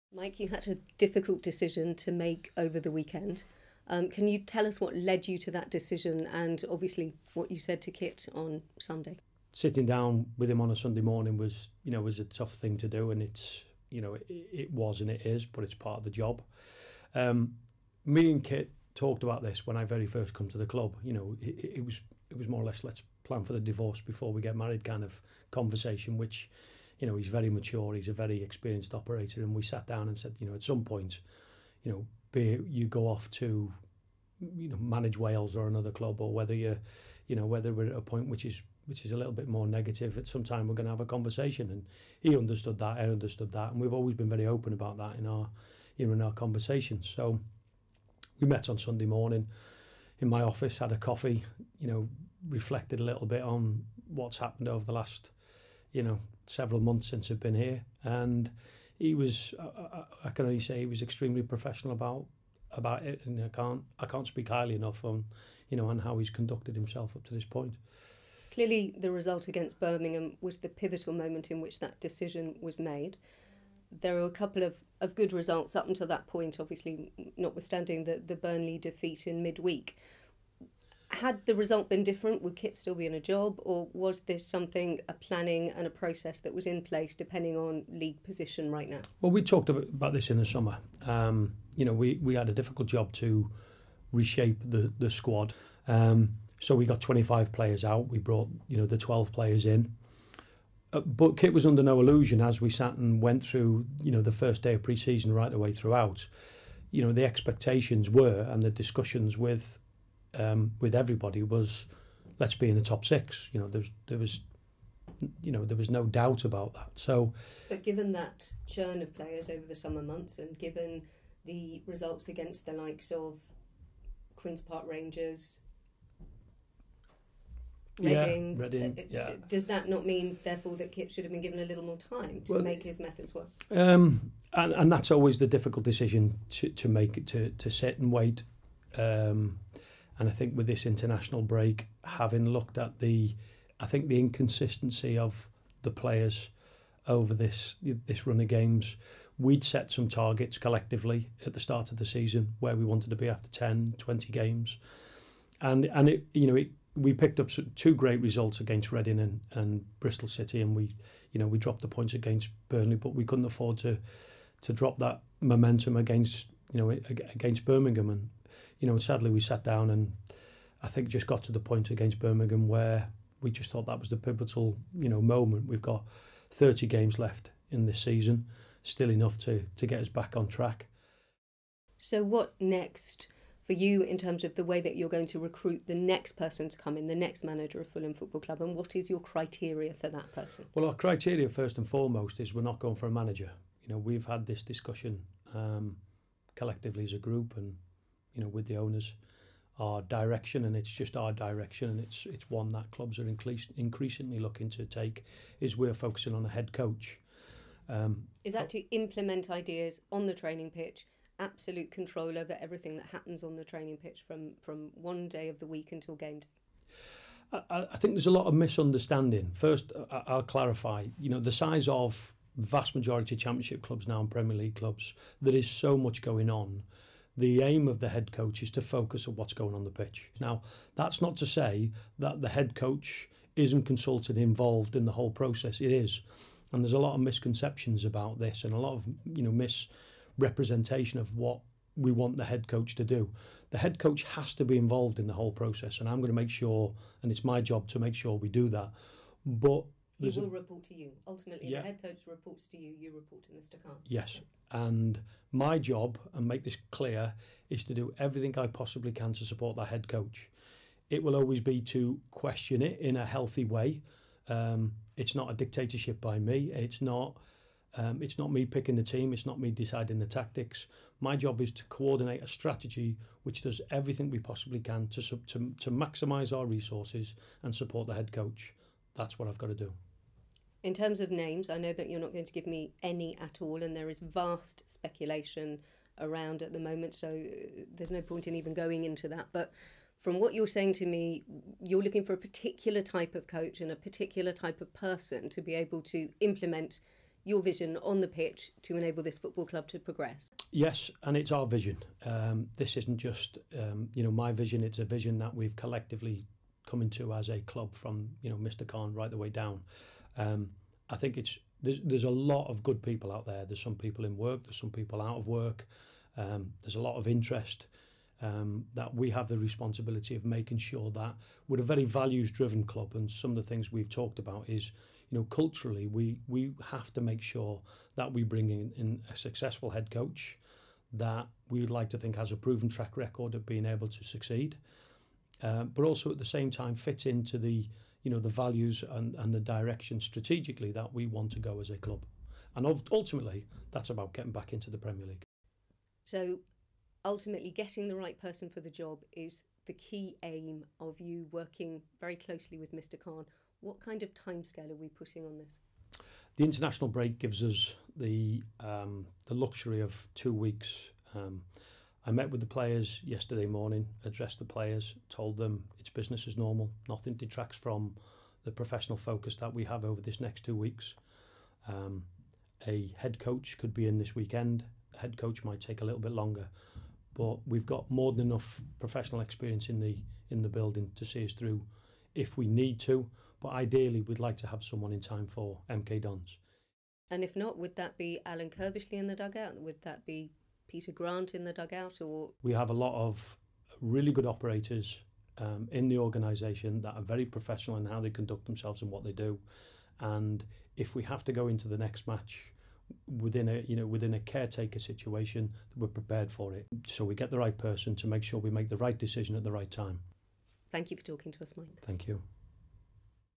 Rigg: Top Six Vision
Chief Football Officer Mike Rigg explains the reasons behind Kit Symons' departure, and discusses what Fulham will be looking for in a Head Coach.